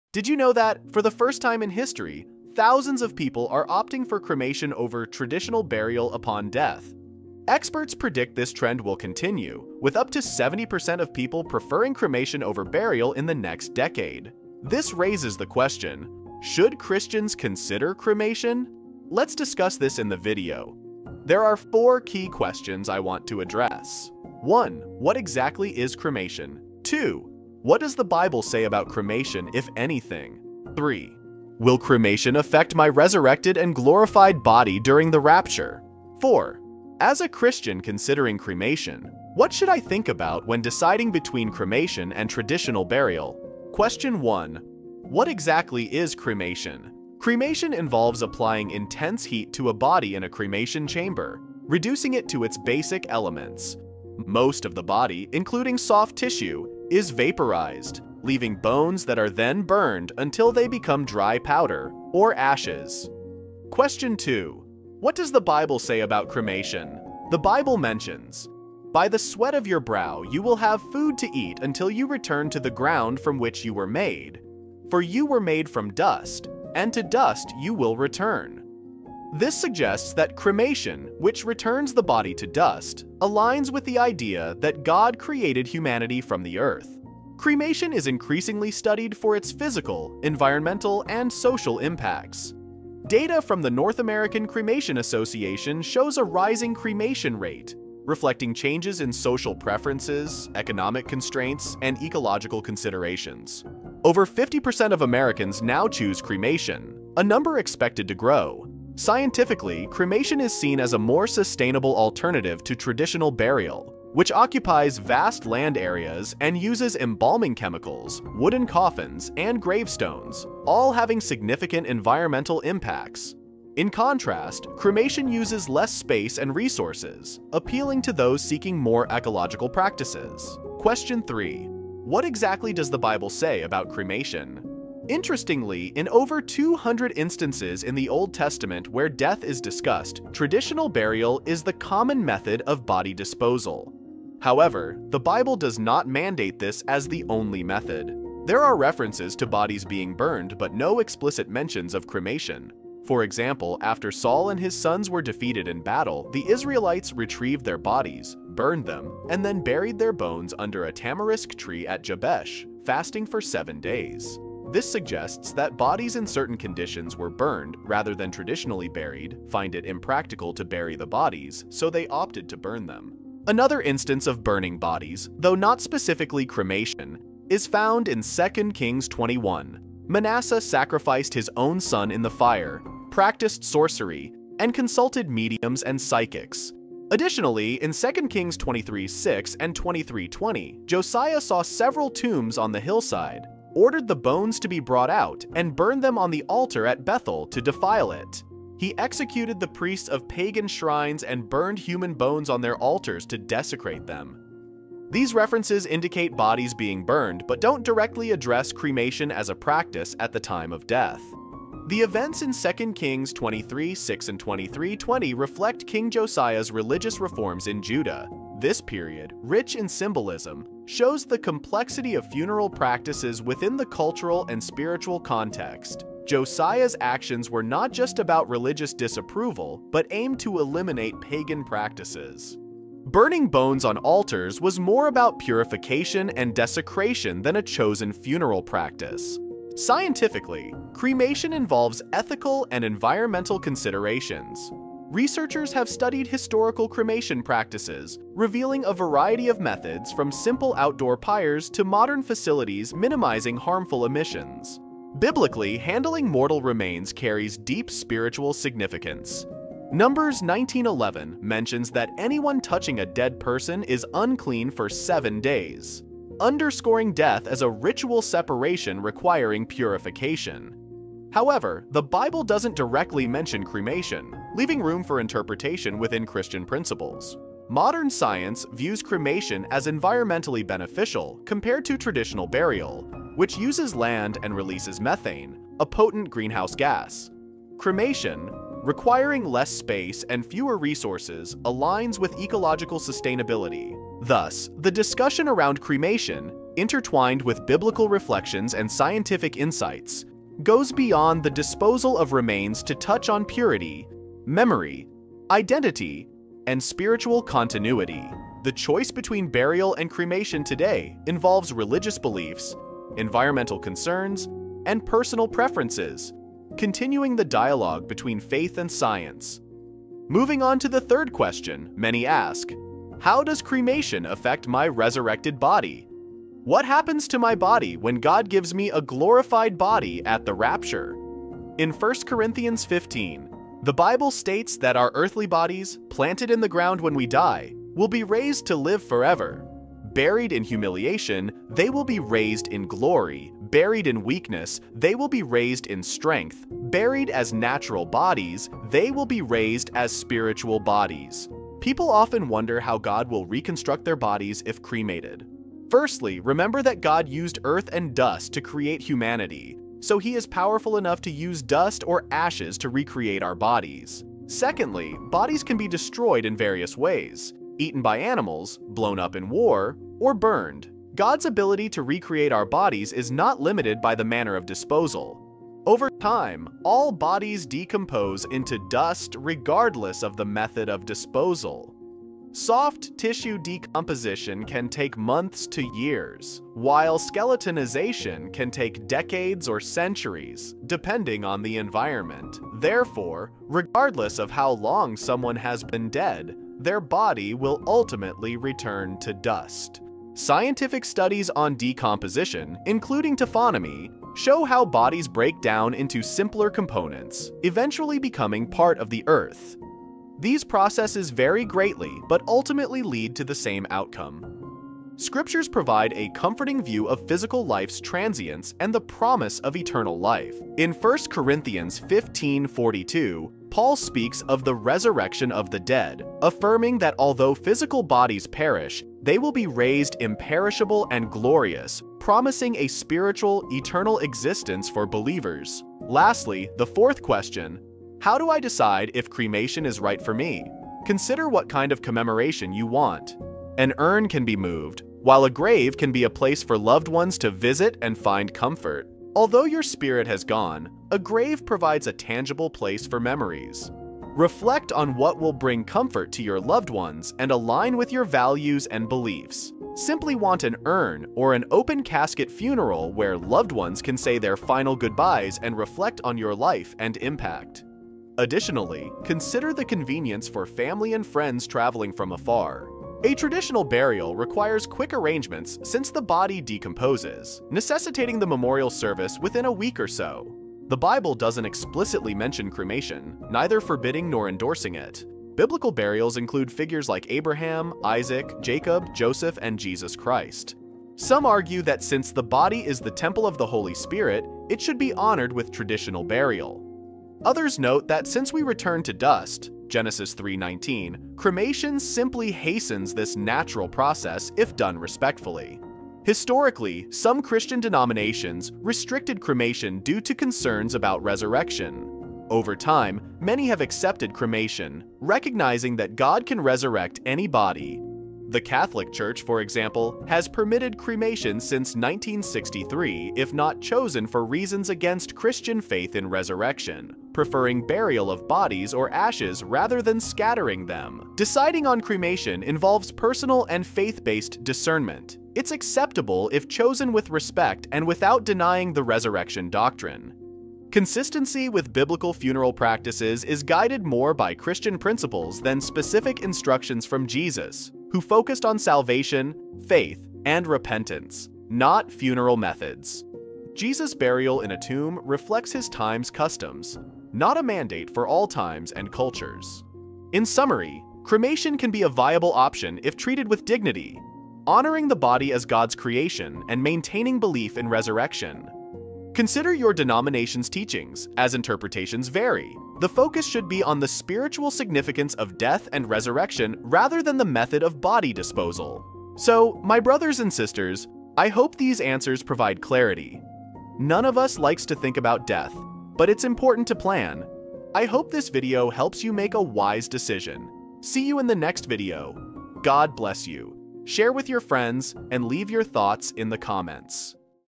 The BIBLE's Surprising Stance on Cremation
In this eye-opening video, we're diving deep into a topic that's been sparking debates among Christians for years: cremation. What does the Bible really say about this increasingly common practice? If you've ever wondered whether cremation aligns with biblical teachings or if it contradicts the faith, this is a must-watch.

As cremation becomes more popular, many are asking whether it fits within the Christian worldview. Does the Bible offer clear guidance on this matter, or is it a gray area left to personal interpretation? This video carefully examines key scriptures and theological perspectives that shed light on the issue. Whether you're considering cremation for yourself or a loved one, or simply curious about the religious implications, this discussion will provide you with essential insights.

Join us as we explore the biblical stance on cremation and what it means for believers today. Don't miss this crucial conversation that could impact how you view life, death, and the afterlife.